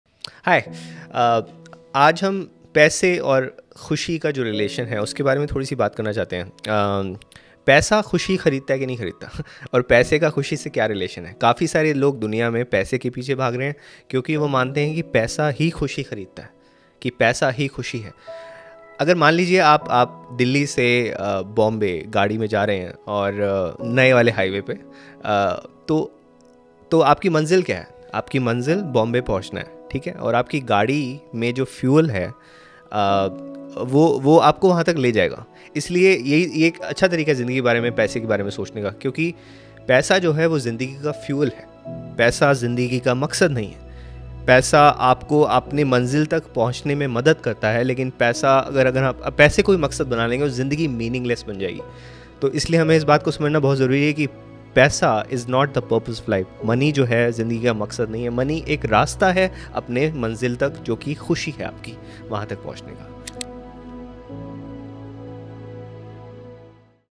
Relationship between money and happiness | Author Amarvani
In this video, we will explore the relationship between money and happiness. What's the importance of money in our happiness?